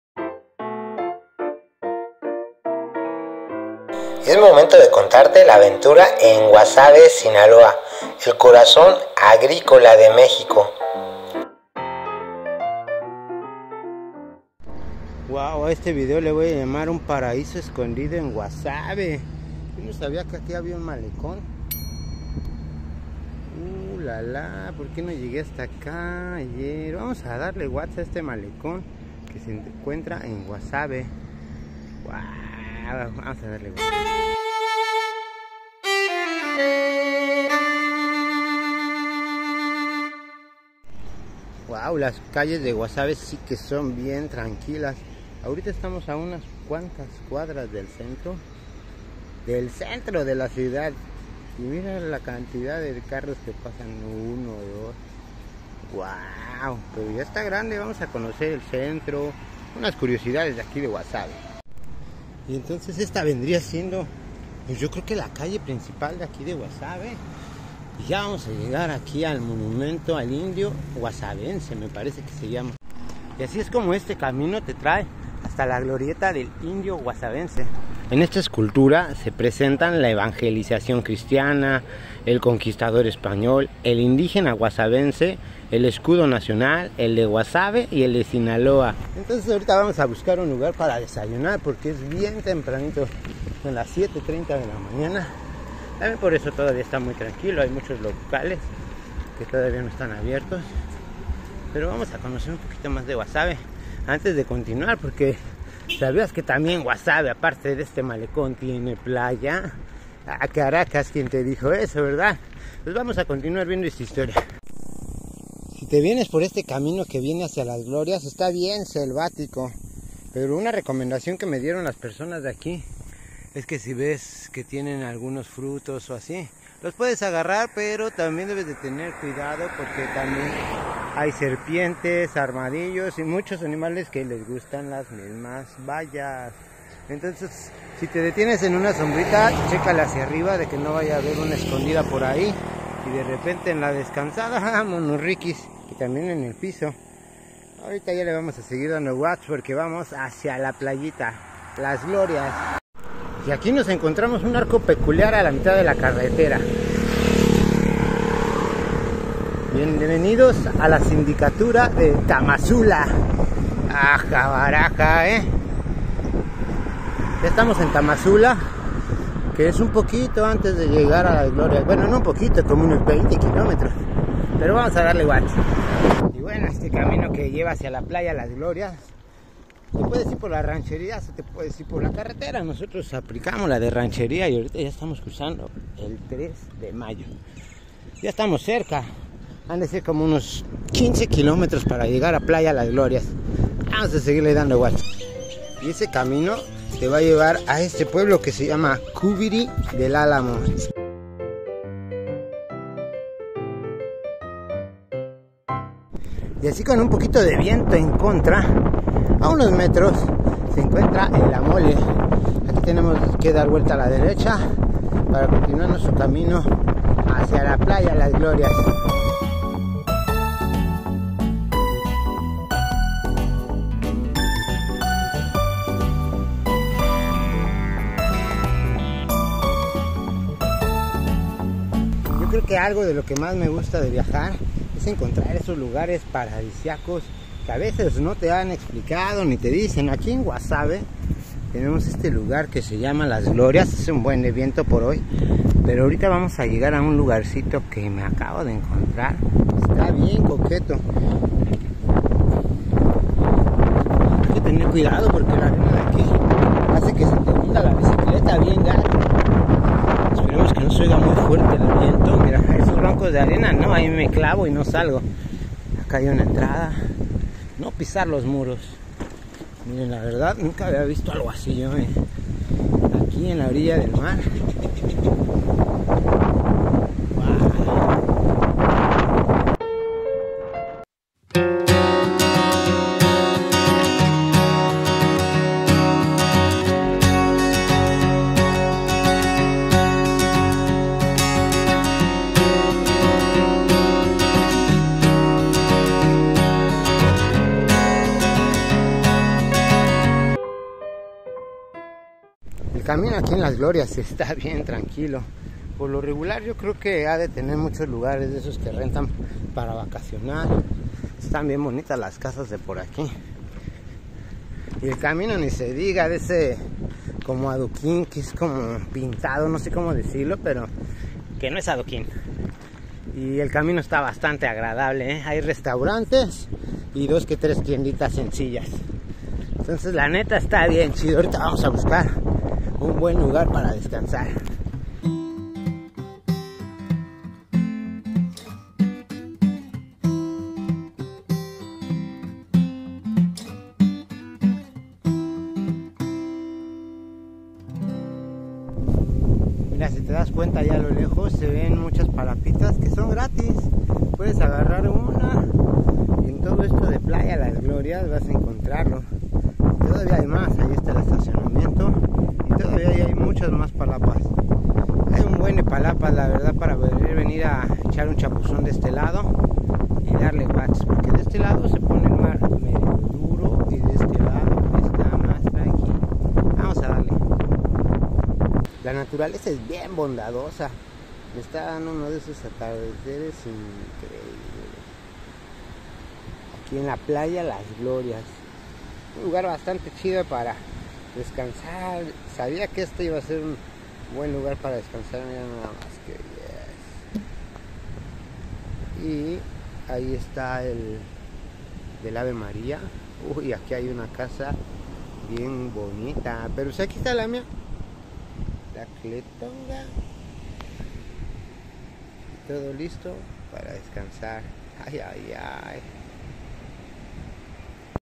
Guasave, Sinaloa " El Corazón Agricola de México"
Guasave, ubicado en el estado de Sinaloa, es reconocido como el corazón agrícola de México. Este vibrante municipio se destaca por su producción agrícola de alta calidad, especialmente en cultivos como el tomate, maíz, frijol y chile. La combinación de tierras fértiles, un clima favorable y una tradición agrícola profundamente arraigada ha permitido a Guasave convertirse en un pilar fundamental del sector agroalimentario del país.

 Además de su riqueza agrícola, Guasave es un lugar de gran belleza natural y cultural, con una comunidad acogedora que celebra sus tradiciones y su patrimonio con orgullo.

en este video encontrarás el camino en bicicleta de Guasave Sinaloa con rumbo a Playa Las Glorias, rancheando llegamos a Tamazula, 3 de mayo y muchos más.

puedes seguir está aventura en tiempo real por instagram